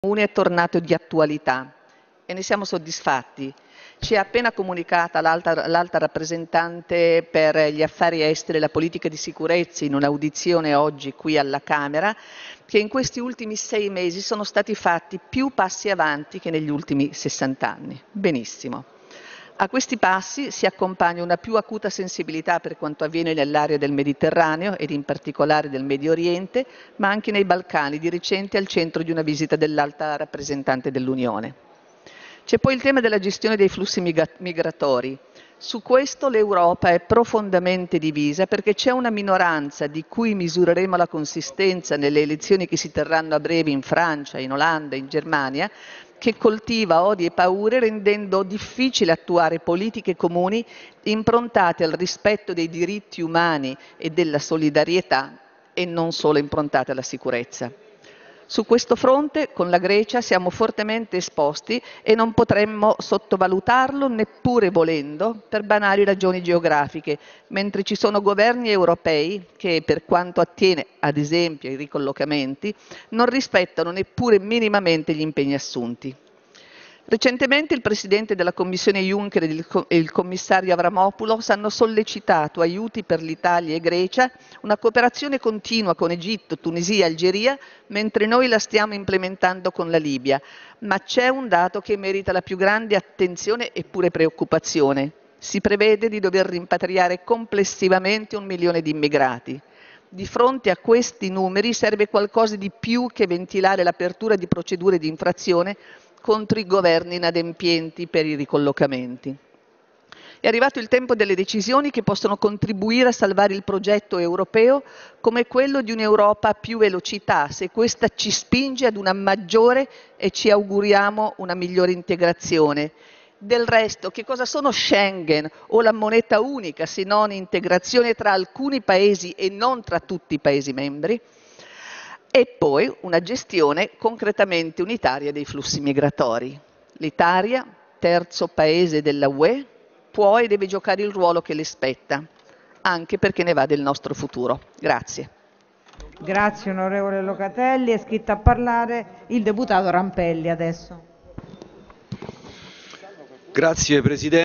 Europa, è il momento delle decisioni
8 marzo 2017 Intervento di Pia Locatelli nel corso del dibattito sulle comunicazioni del Presidente Gentiloni in vista del Consiglio europeo